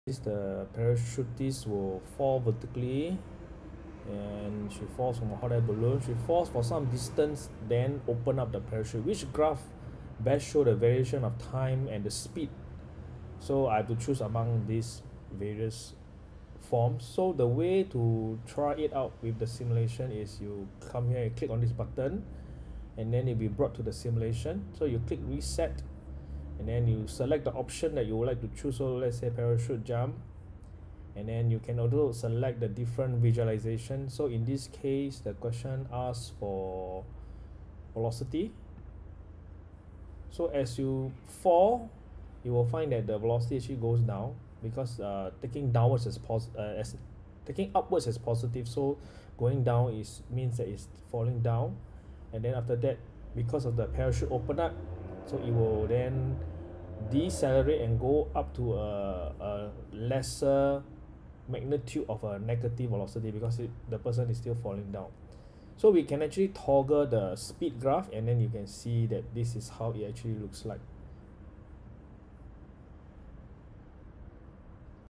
Q1 Which graph best shows the variation with time t of the speed v of the parachutist?
A parachutist falls vertically from rest at time t = 0 from a hot-air balloon. She falls for some distance before opening the parachute.

Which graph best shows the variation with time t of the speed v of the parachutist?